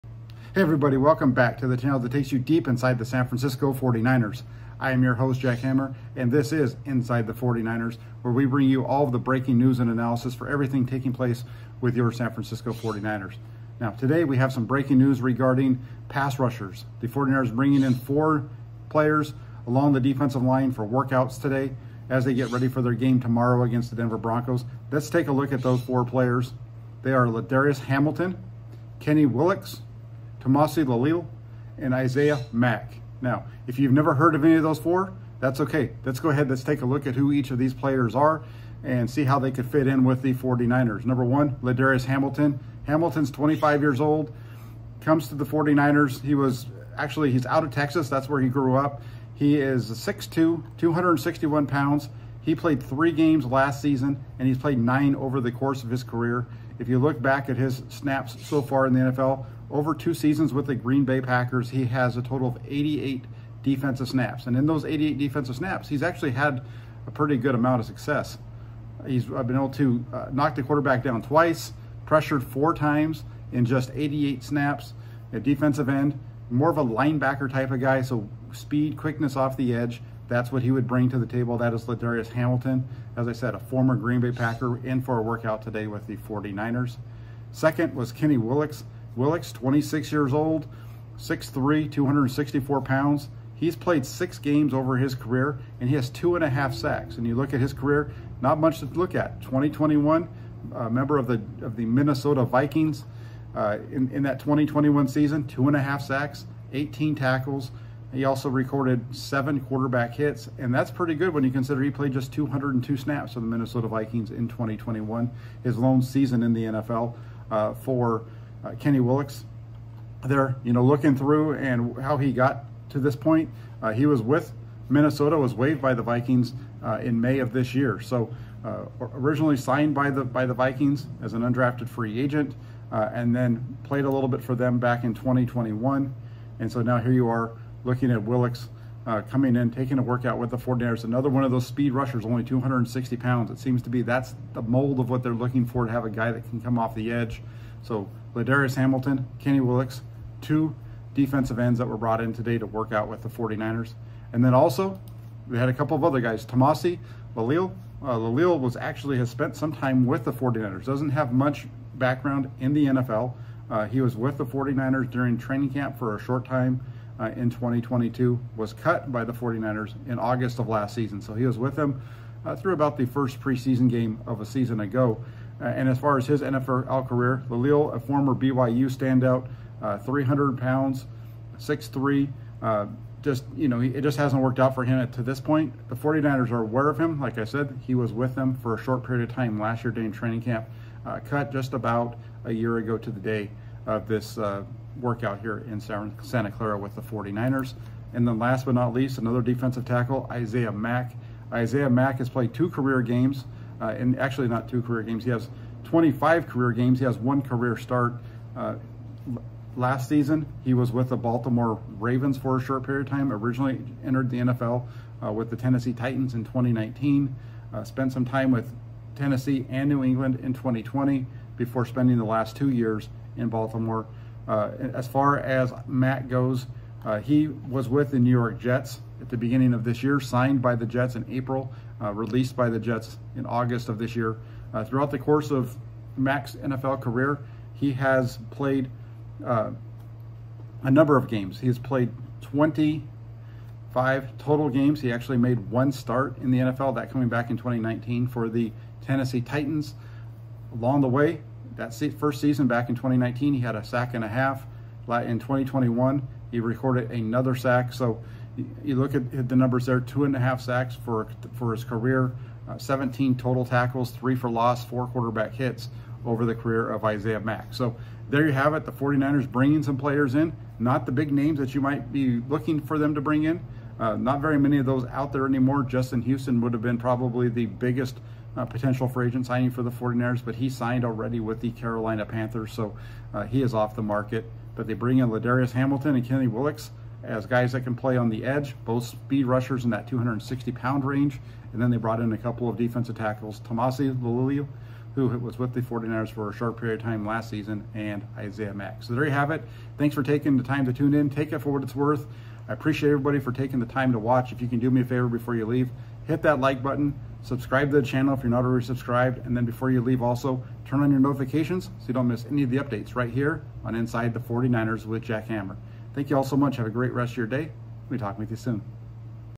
🚨INSIDE SCOOP: 49ERS LOOK TO BOLSTER PASS RUSH🚨
INSIDE SCOOP: 49ERS LOOK TO BOLSTER PASS RUSH. BRING IN FOUR DEFENSIVE LINEMEN FOR WORKOUTS.